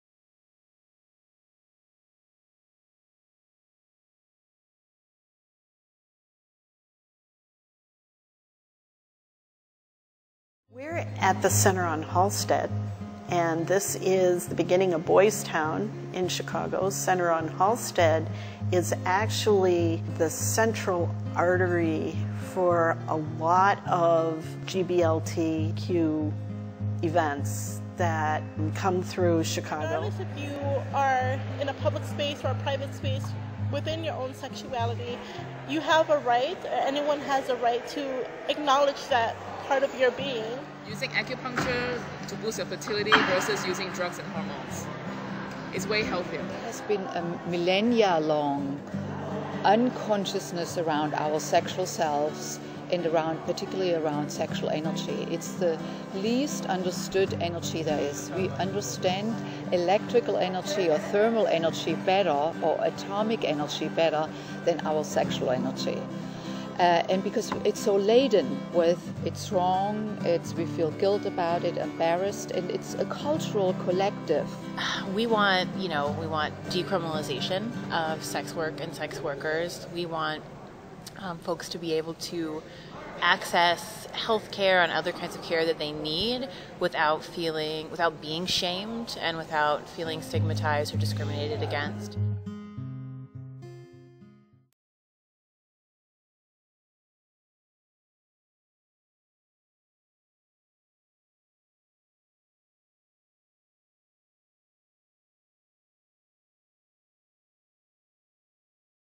SEX Education: Expert's Testimony
Every problem we have around Gender & Sexuality illustrates how human judgment is not a simple mental process, but also engages our emotions and bodies. We need to create a forum to listen and respond to the great many influences which contribute to our opinions and cultural standards.  Though we are looking for academics and professionals in various industries like Social Sciences, Political Sciences, Education, Advertising, Public Relations, Marketing, Business Management, Law and Public Speaking, to make our efforts the most successful, we would have to include entertainment celebrities, athletes as well.  Adding dimension to the spectrum will allow for a comprehensive approach to the already existing conservative push toward what is presented in a Sex/Gender Education and Awareness.

With all the information available to us, hopefully we will engender a comprehensive Education and Awareness campaign.  Our main focus will be the marketing and promotion of inspiring information from these multifaceted events that are scheduled to begin the week after Chicago's annual Slut Walk.. SLUT WALK is a feminist rights movement which started in Toronto Canada on January 24, 2011, when a Toronto Policeman remarked: "women should avoid dressing like sluts in order not to be victimized." This shocking statement ignited a protest here in Chicago so big it's on its 3rd consecutive year.  We are aiming to take advantage of the momentum and the public's receptivity of this topic to begin our campaign.